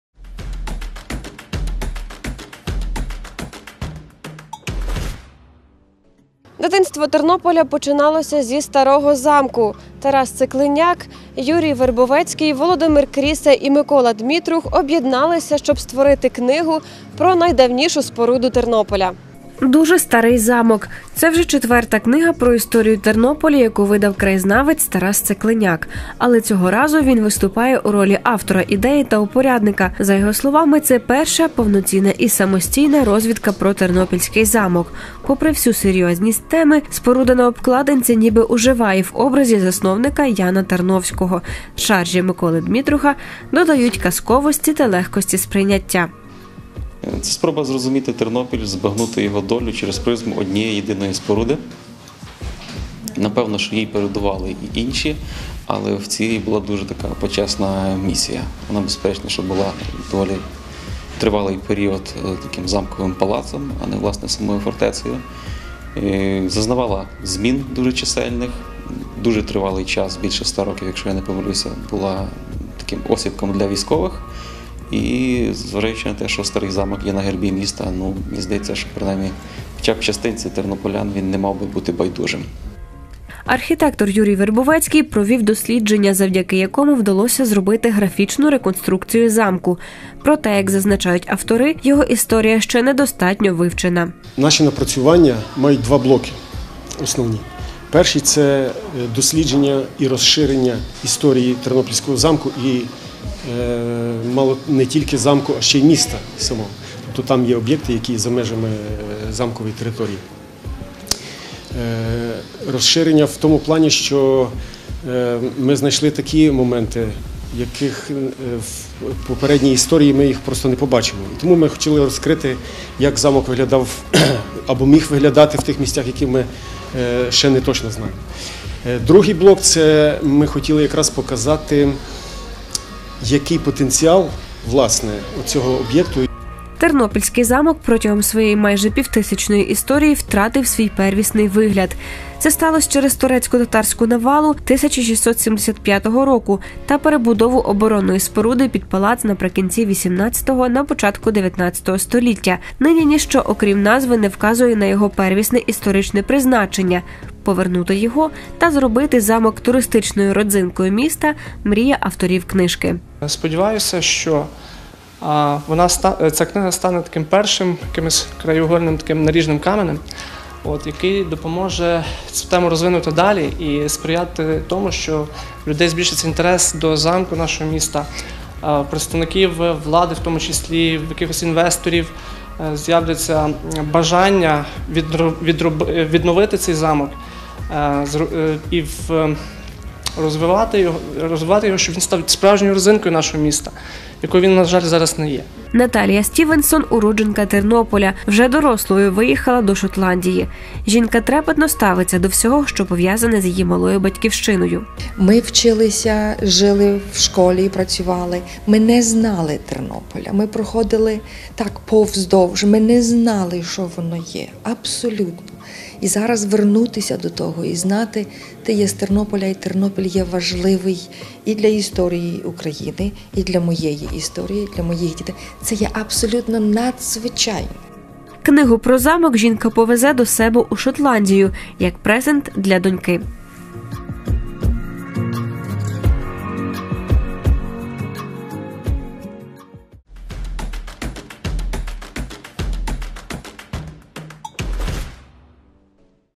Сюжет - "Дуже старий замок"
Замок – одна із візитівок Тернополя, якою милуються тисячі туристів.  Він приховує чимало секретів і таємниць про які дізнавалася наша колега Лілія Лобур.